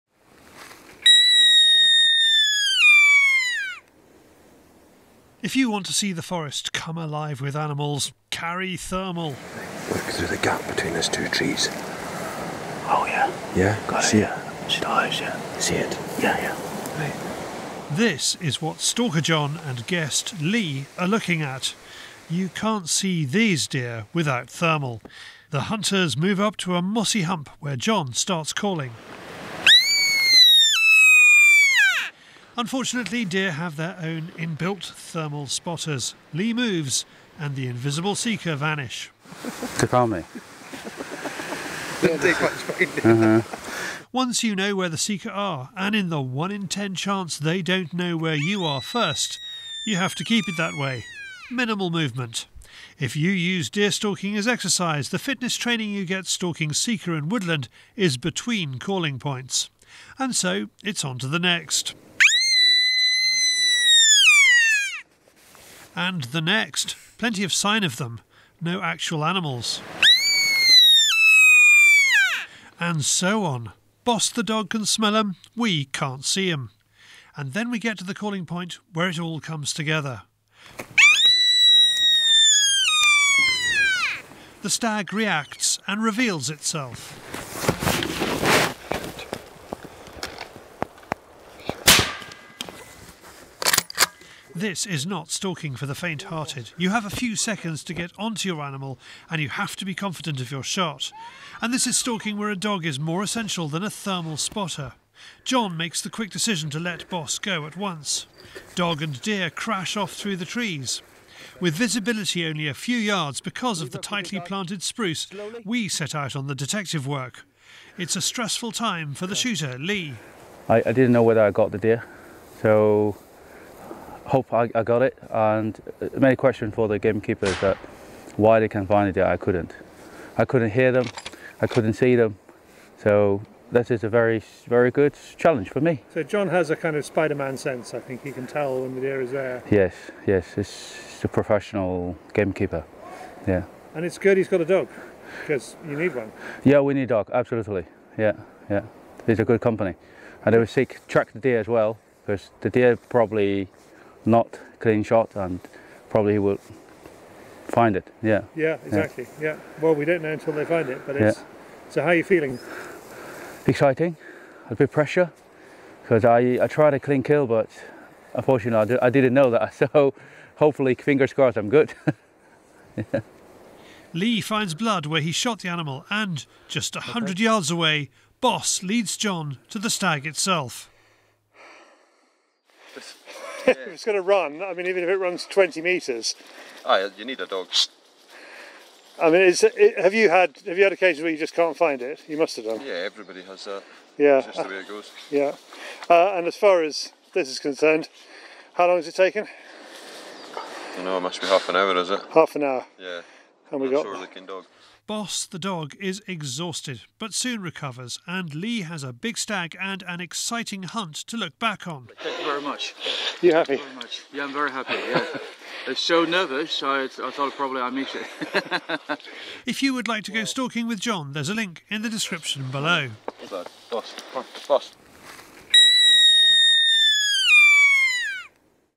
How to call in sika stags
You pack your rifle. If you pack two other vital accessories for woodland sika stalking, they are a thermal spotter and a good dog. One’s to find the animal when it’s alive, the other to find it after you have shot it. It’s the sika rut in North-West Scotland. Deer manager John Dodd takes He Li from Shooterking out after a stag.